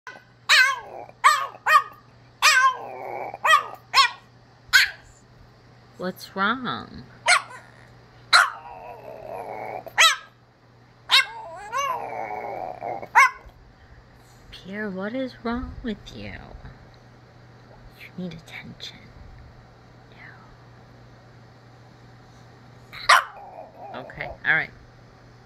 Frenchie blue Pierre monkey howling for attention
French Bulldog being needy